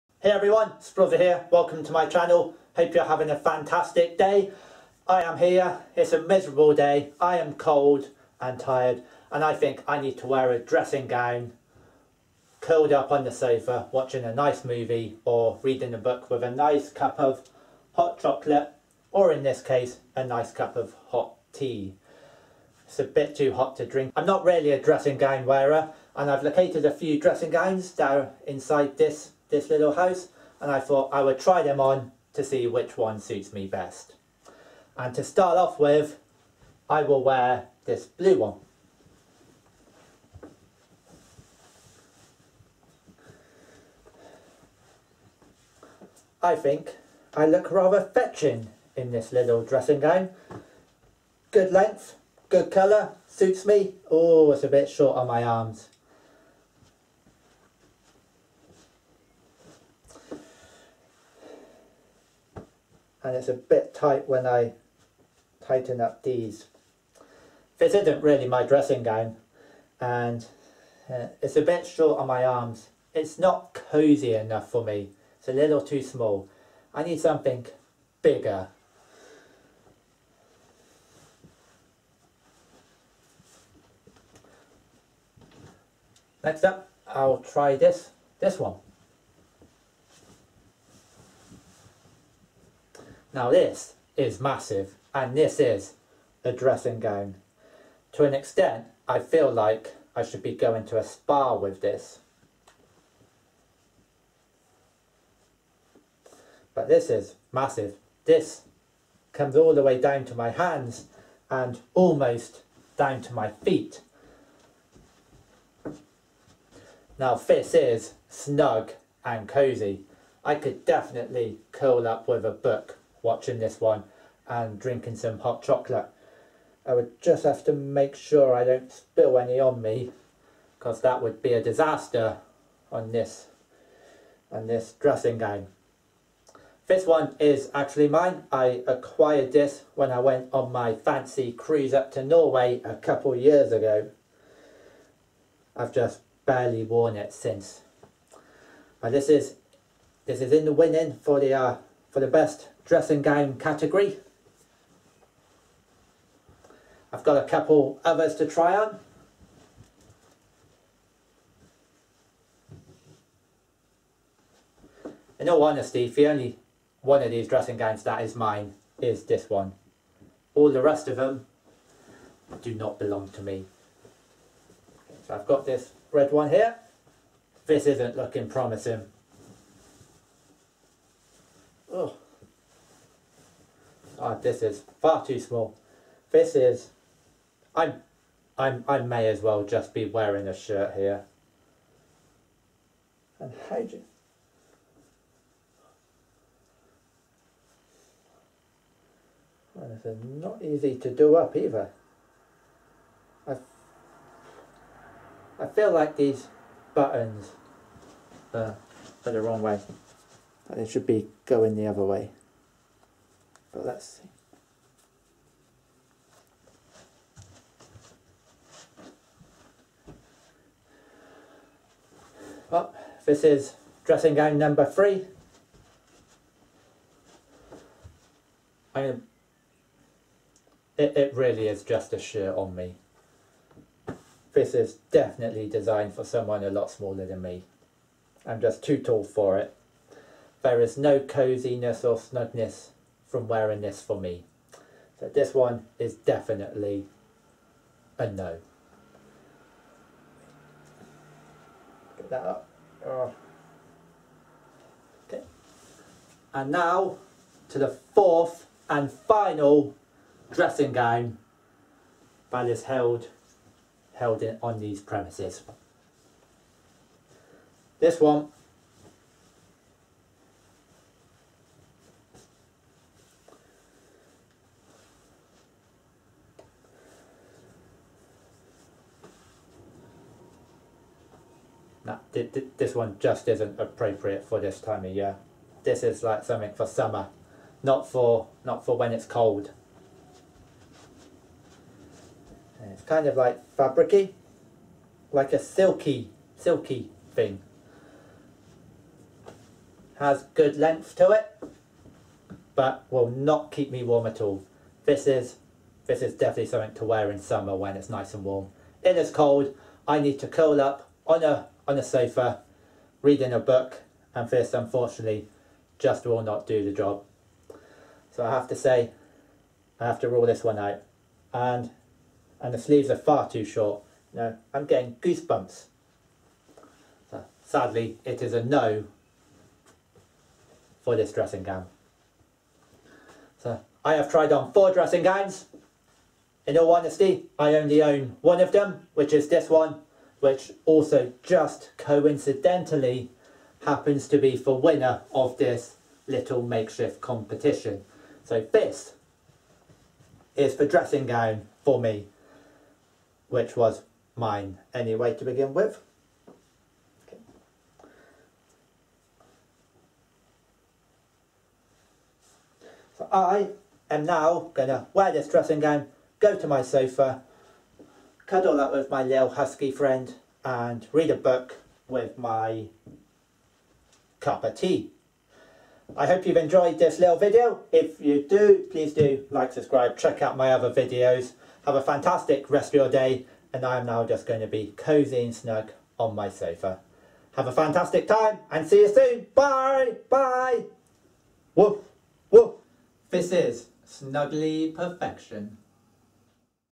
Dressing Gowns Try On
I try on a few dressing gowns for the fun of it.